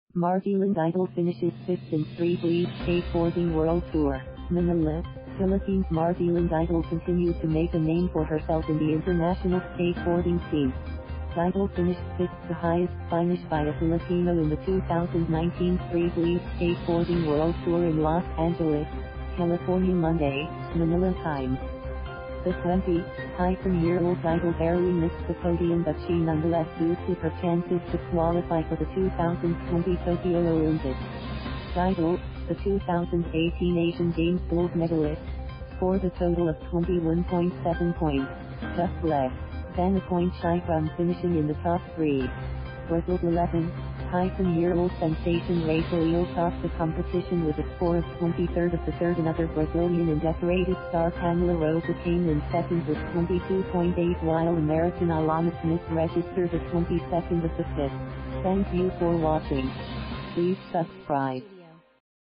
Margielyn Didal finishes 5th in Street League Skateboarding World Tour
MANILA, Philippines–Margielyn Didal continued to make a name for herself in the international skateboarding scene. Didal finished fifth—the highest finish by a Filipino—in the 2019 Street League Skateboarding World Tour in Los Angeles, California Monday (Manila time). The 20-year-old Didal barely missed the podium but she nonetheless boosted her chances to qualify for the 2020 Tokyo Olympics. Didal, the 2018 Asian Games gold medalist, scored a total of 21.7 points, just less than a point shy from finishing in the top three. Brazil’s 11-year-old sensation Rayssa Leal topped the competition with a score of 23.3. Another Brazilian in decorated star Pamela Rosa came in second with 22.8 while American Alana Smith registered a 22.5.